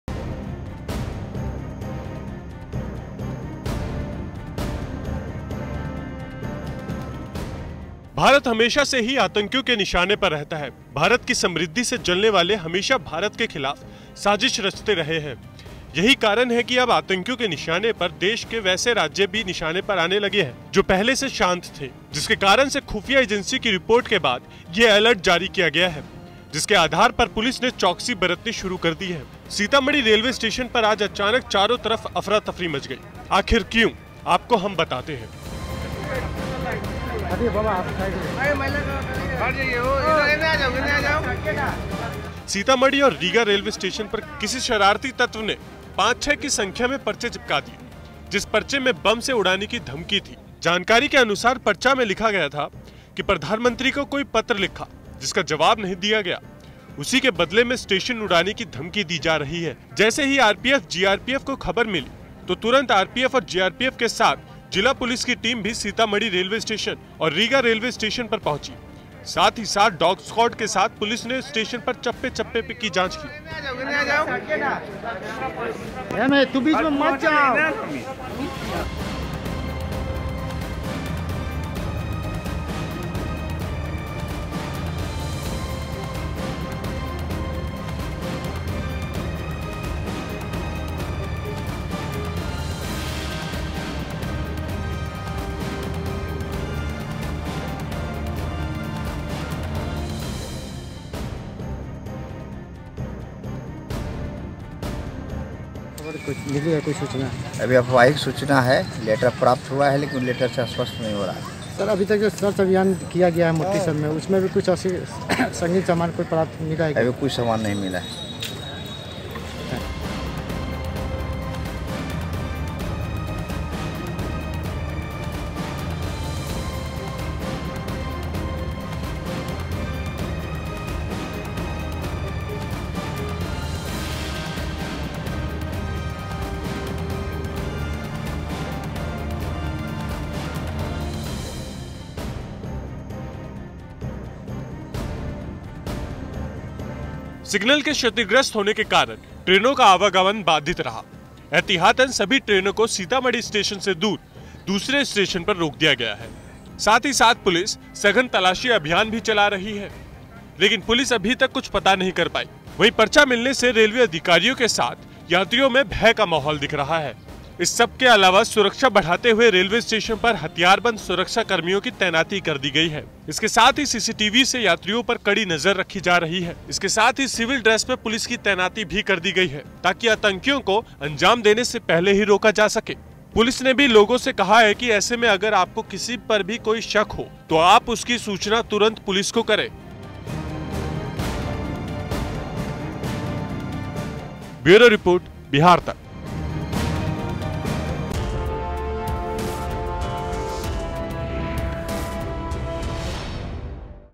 Bihar में High Alert , देखिए Sitamarhi Station पर क्या हुआ ? | Bihar Tak
भारत हमेशा से ही आंतकियों के निशाने पर रहता है । भारत की समृद्धि से जलने वाले हमेशा भारत के खिलाफ साजिश रचते रहे है,